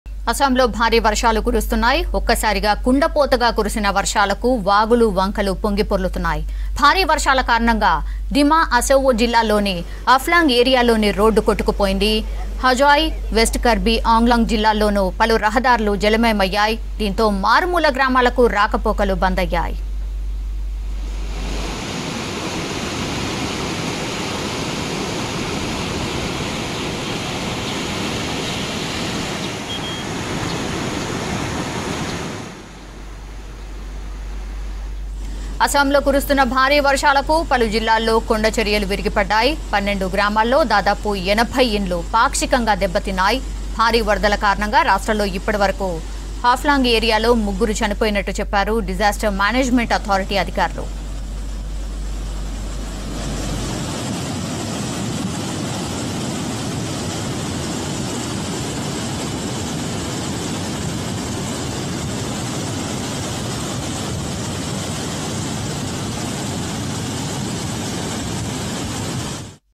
Heavy Rain Lashes In Assam, Roads Submerged In Rain Water | V6 News
మనల్ని ప్రశ్నిస్తుండ్రు | రాజ్యసభకు తాత నామినేషన్